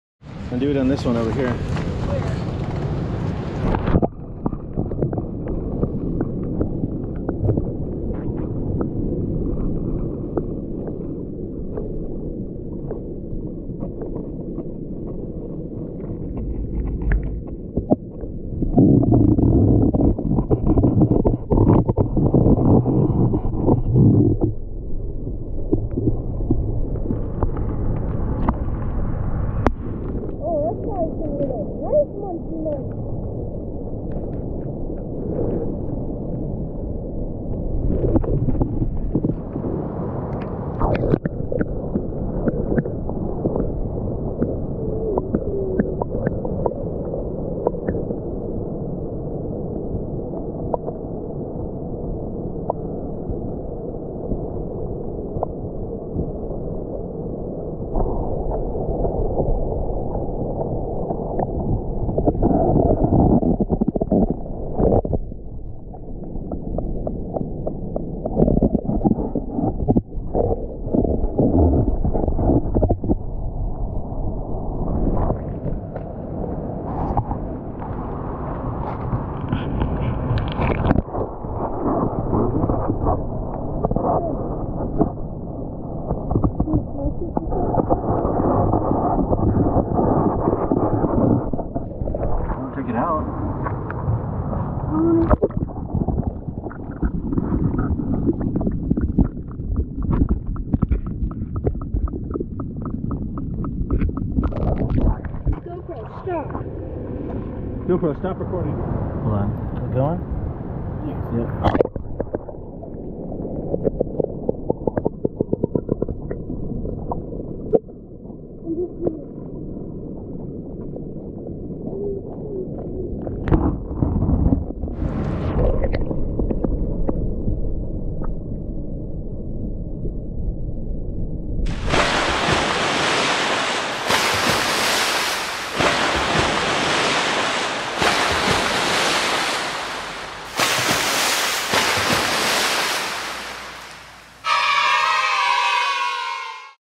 Hobbit Beach Oregon Tide Pools
The sea anemones were hungry for mussels